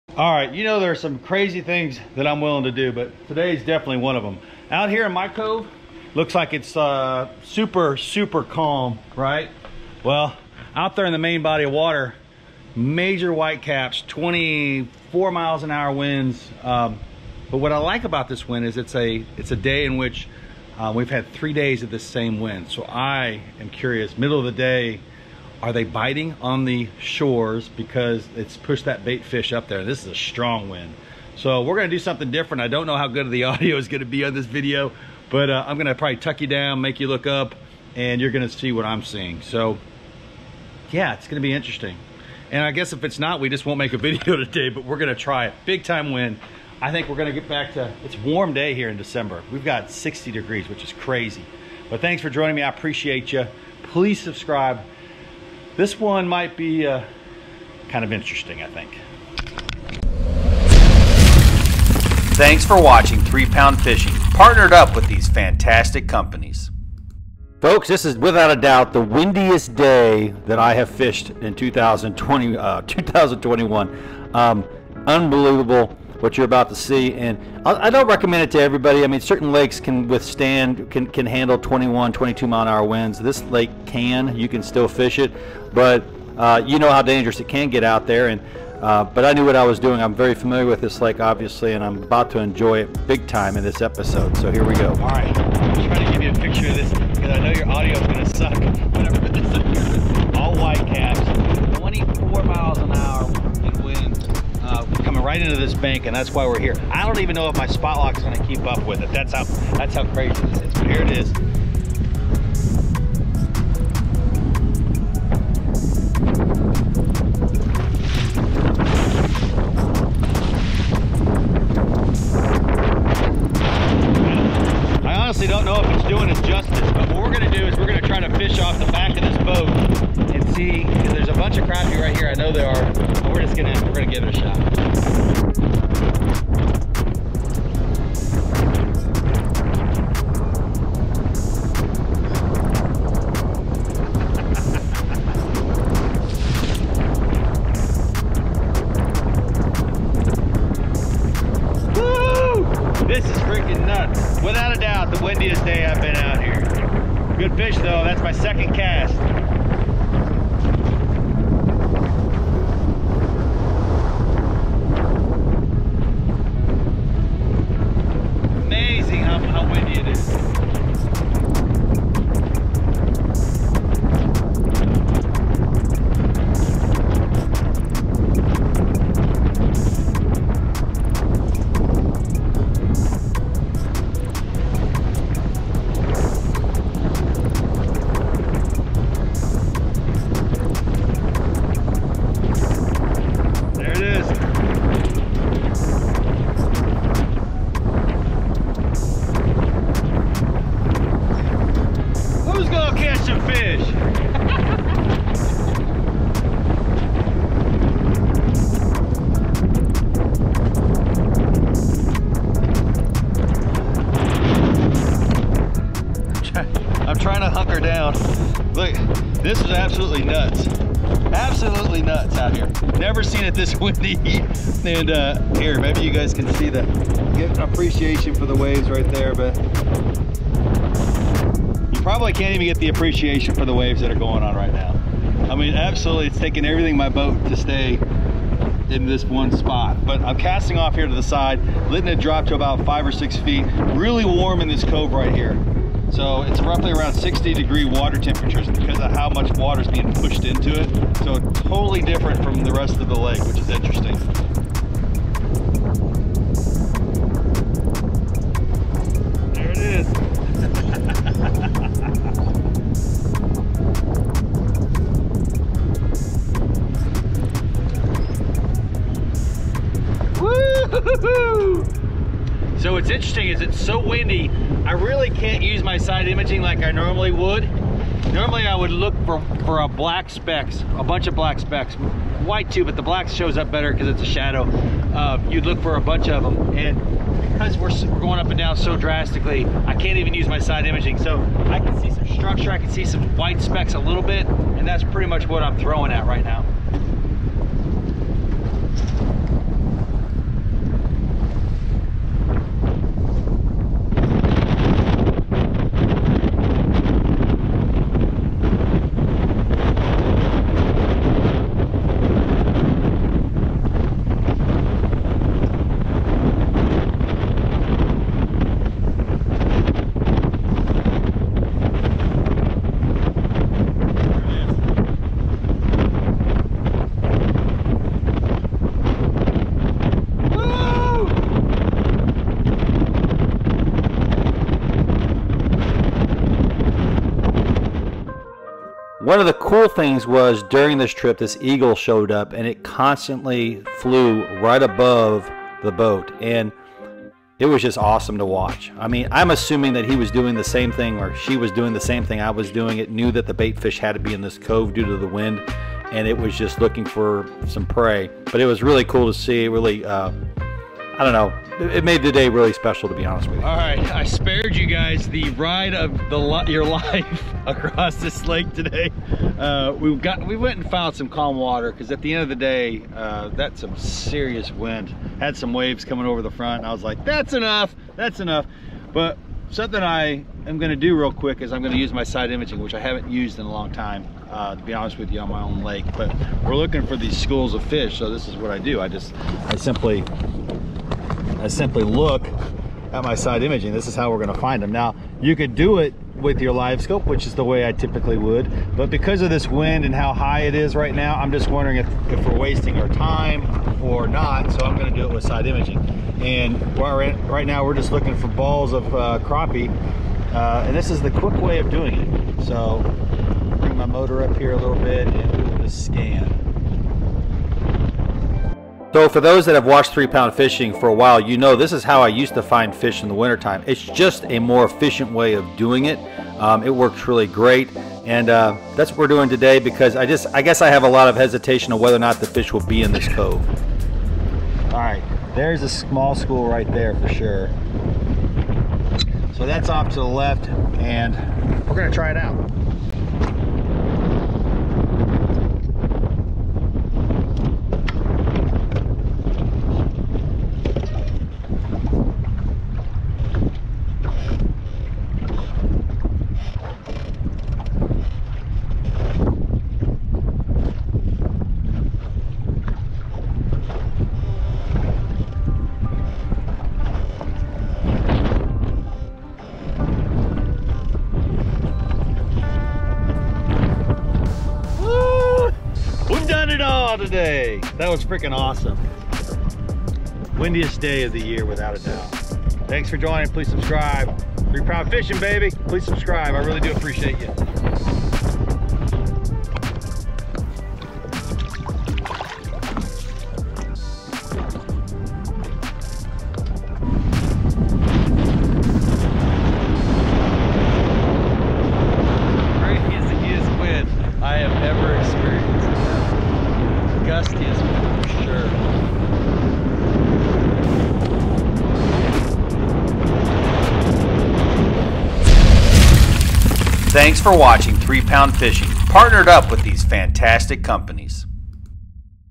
Are you a Diehard Crappie Angler?
Only the serious need to watch this. Crazy windy, crazy hard. lets do this diehards!

For more information on 3 Pound Fishing and GUIDE TRIPS go